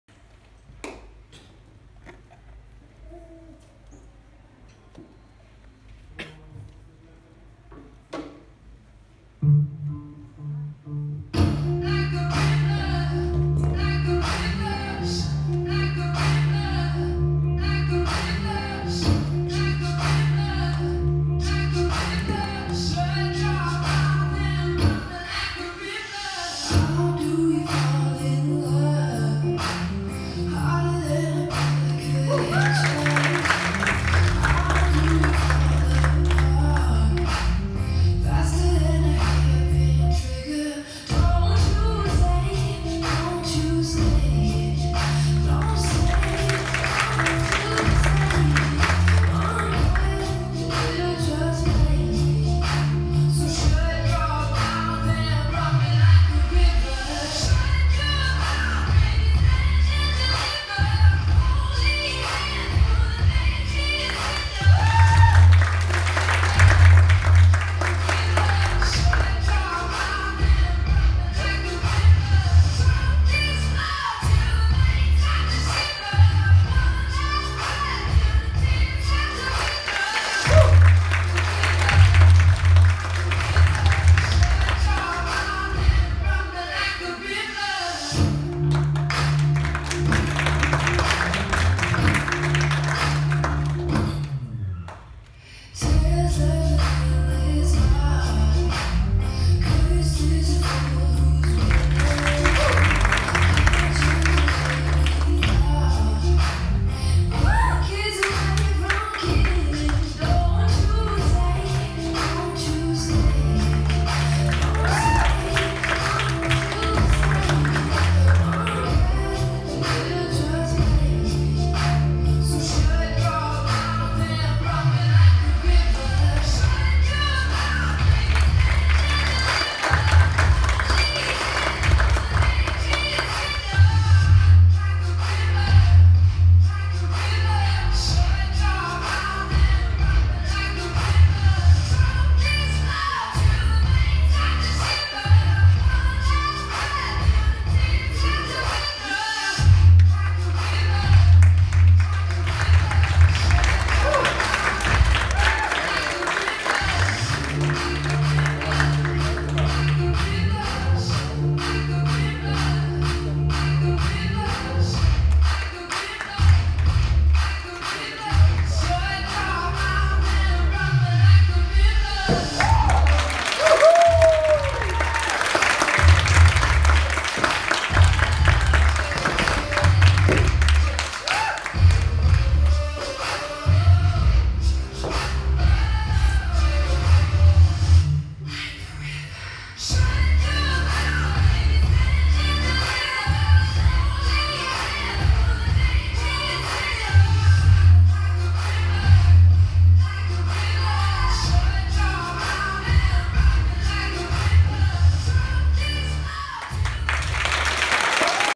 Zakončení jarního semestru 2019 - nová lektorka - Terka Jeníčková
Od nového semestru 9/2019 u nás začne učit tato nadaná mladá a zkušená lektorka Terka Jeníčková!
Těšíme se na její skvělé lekce Aerial Hoop pro pokročilé!